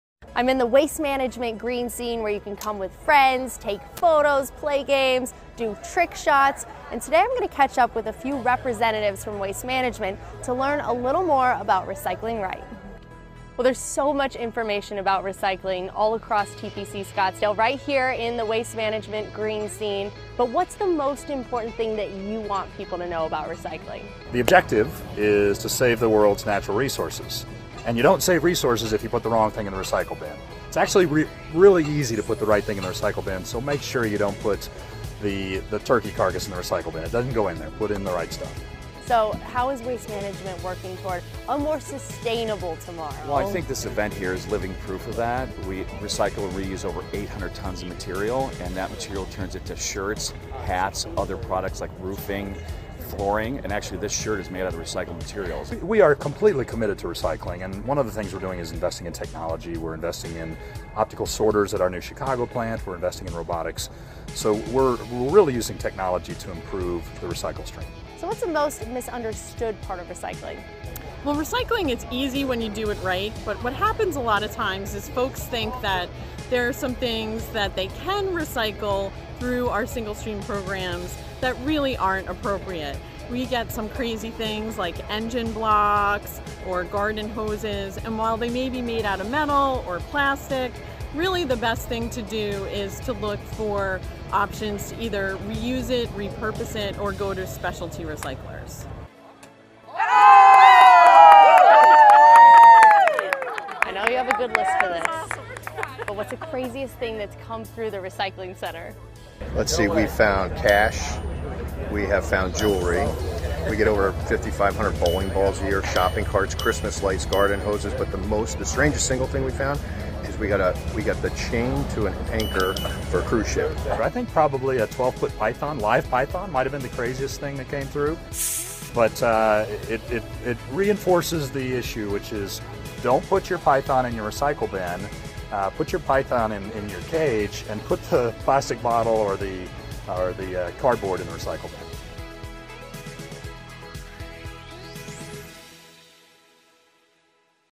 Recycling Education at the WM Green Scene ft. Alexandra O’Laughlin
Golf Channel’s Alexandra O’Laughlin learns how to recycle right at Waste Management’s own interactive recycling experience.